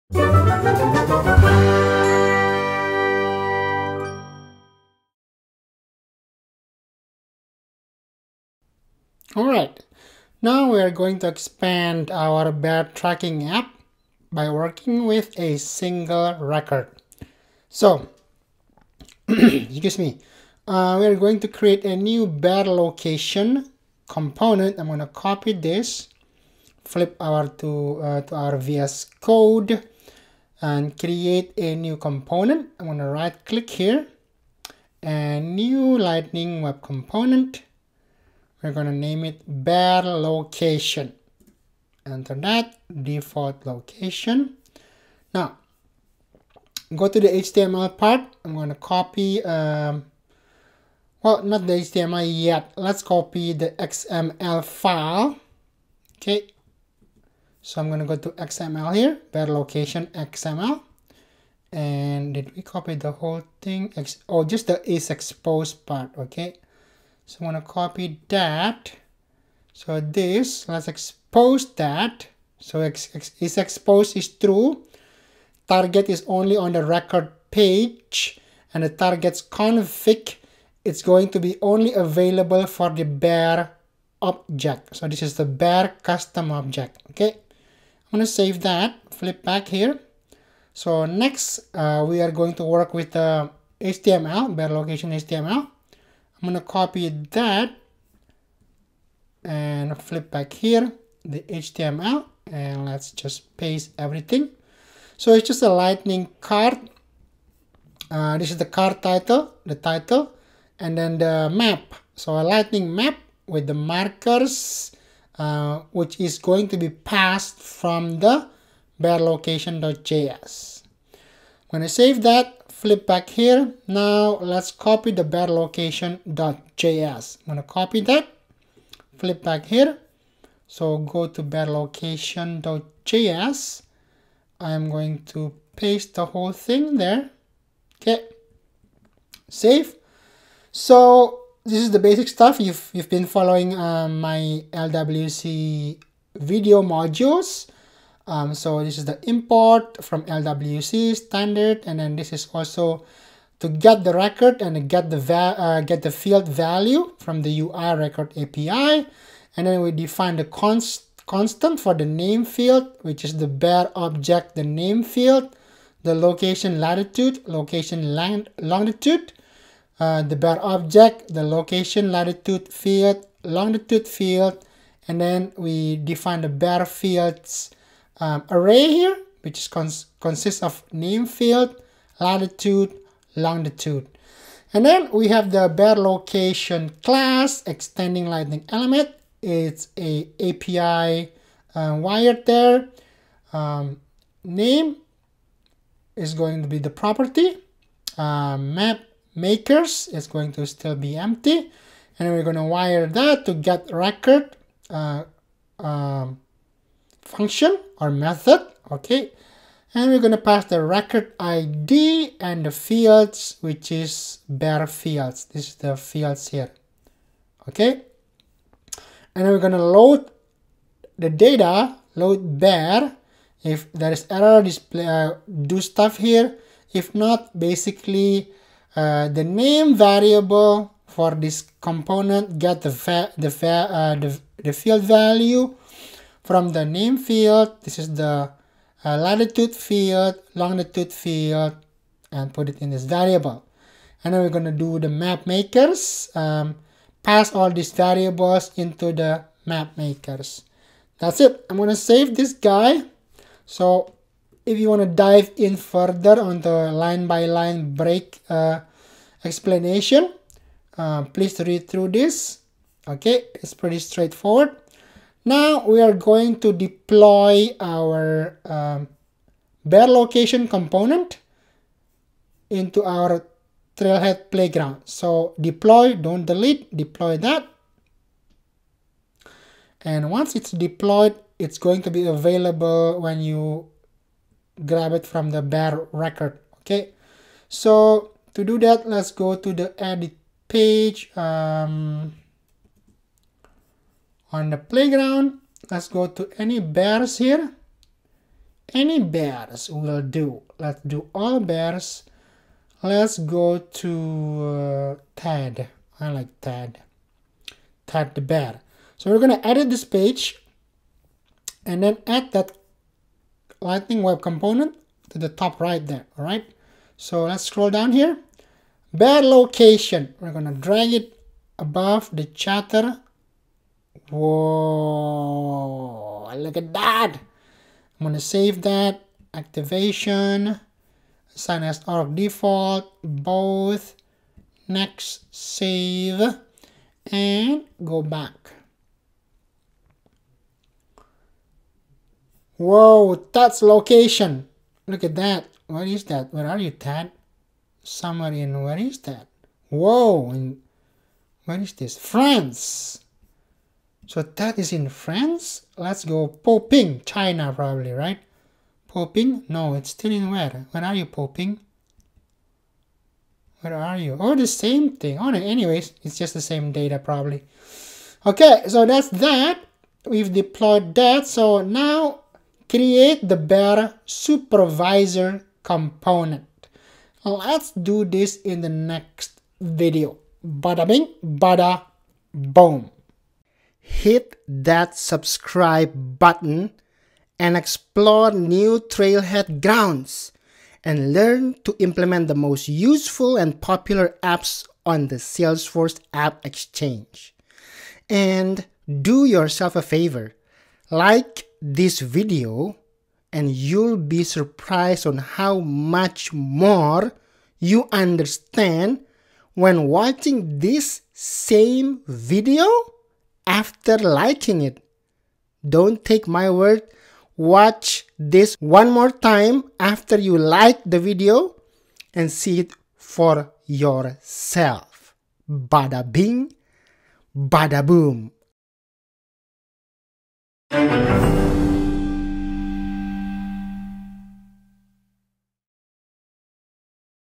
LWC - Work with a Single Record 1/2
What You’ll Do
Ursus park rangers need your help to track down bears that wander in the park. They already have some information entered in Salesforce, but they need you to provide them with a customized app experience.

Create the Bear Location Component